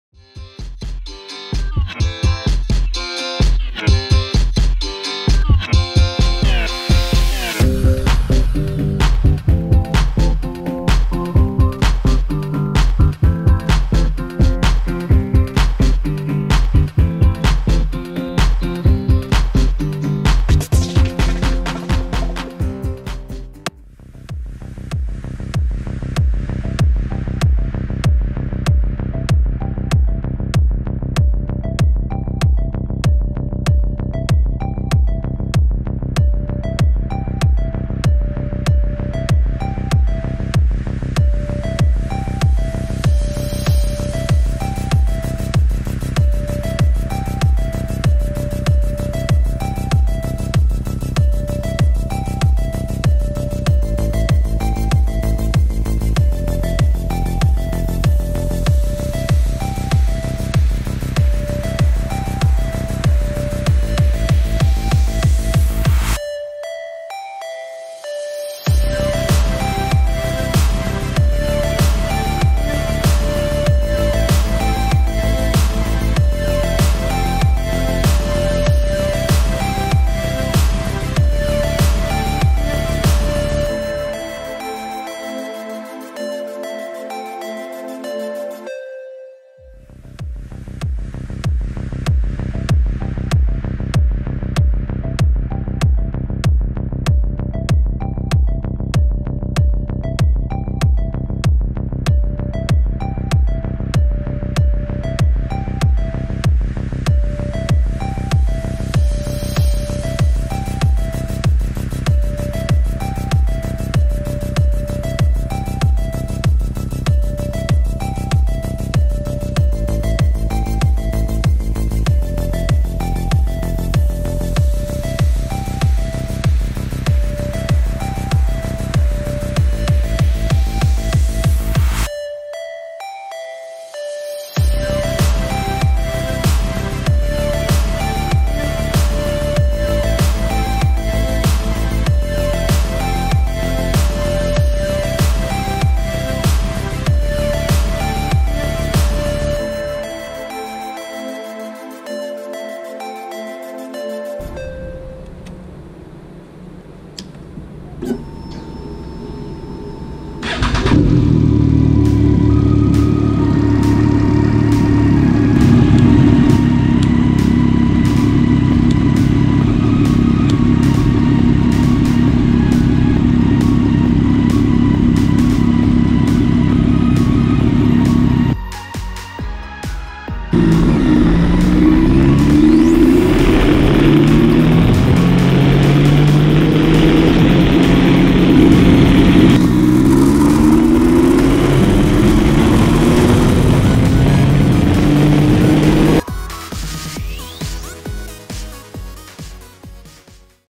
How to Auxbeam on Triumph Street Triple - Tiny Jen
Bike model : Triumph Street Triple 675r
Auxbeam LED light bulbs size : H4